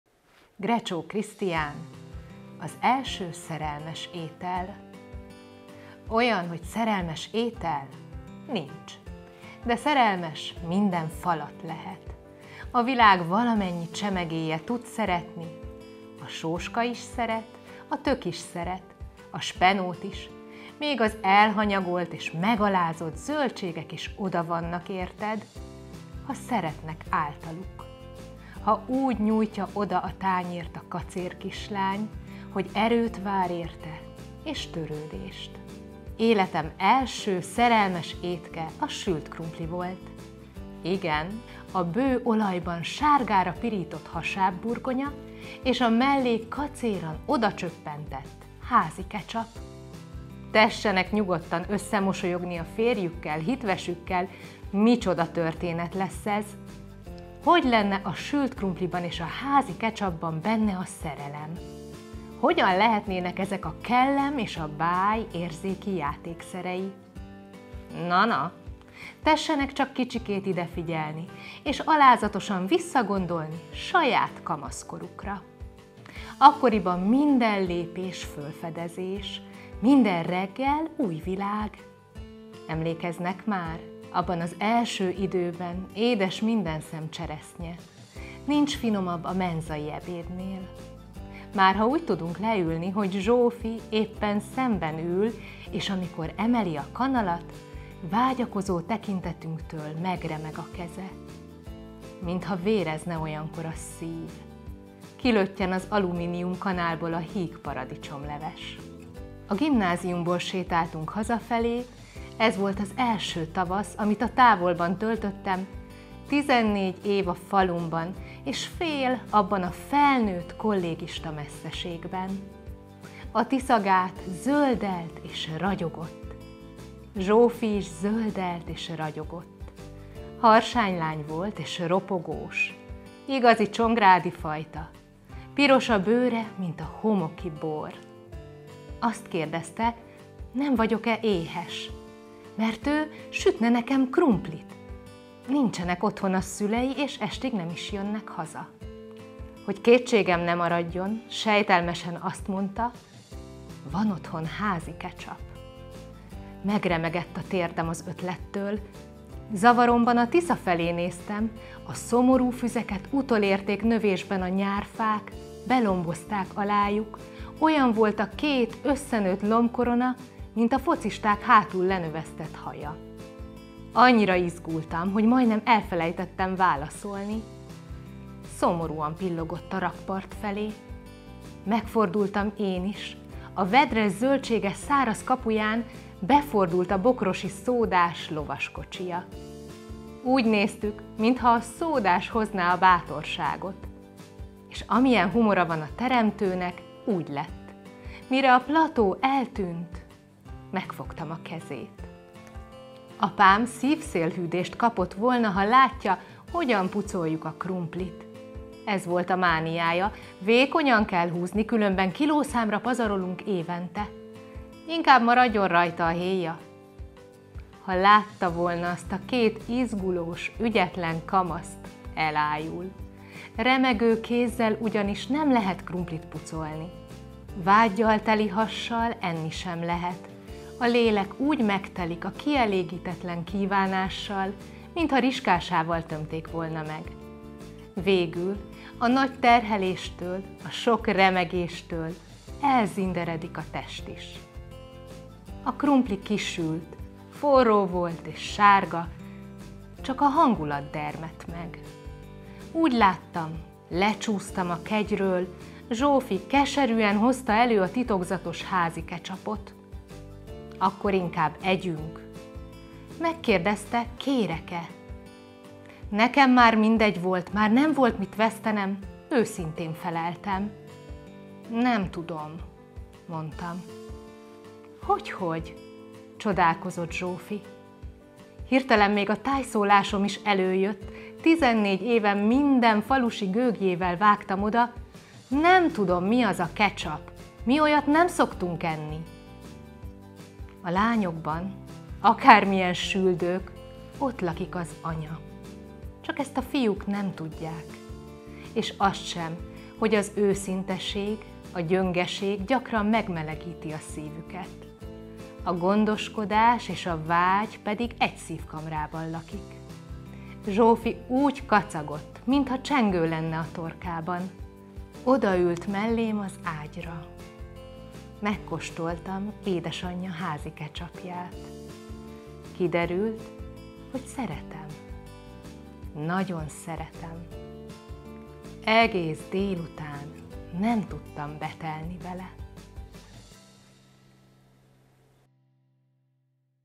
Grecsó Krisztián: Az első szerelmes étel
Egy történet nagyoknak! De milyen!❤️
Emlékszik még mindenki milyen szerelmesnek lenni? Sokan éppen azok? Nagyon reméljük. Hiszen talán nincs is csodálatosabb, mint egy kezdeti szerelem ártatlan, remegtető érzései. A hosszú pillantások, amik felrebbentik a pillangókat a hasban. Az izgulós sutaság, melyet felold egy kiadós nevetés. A remény édes íze, hogy most szerethetünk és szeretve leszünk.
Sokszor méltattuk már Grecsó Krisztiánt. Úgy játszik a szavakkal ebben a történetben is, hogy a hallgató azt érezheti róla ír. Olyan egyszerűen és hétköznapian, olyan Grecsósan ragadja meg a témát, hogy az utánozhatatlan. És egyszerűségében csodaszép.❤️

Mesélő: Vörös Katalin
Videó: Hokedli Stúdió - Tóth Dani